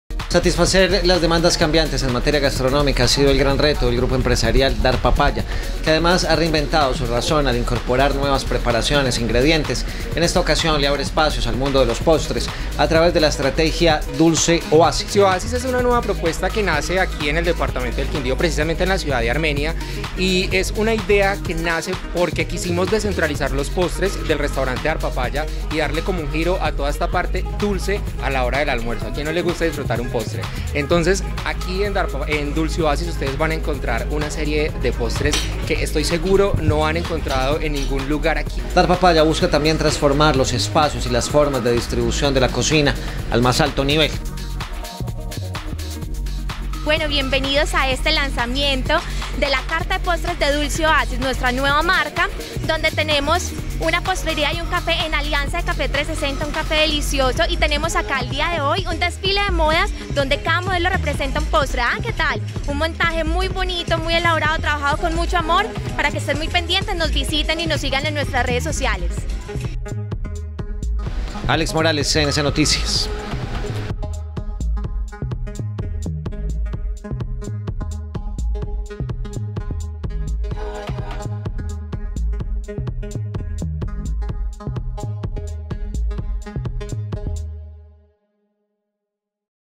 Carta postre "Dulce Oasis"
Satisfacer las demandas cambiantes en materia gastronómica
ha sido el gran reto del grupo empresarial Darpapaya , que además
han  reinventado su razón de ser al incorporar nuevas.
Preparaciones e ingredientes, en está ocasión  abre espacios al mundo de los postres .
 A través de la estrategia "Dulce Oasis"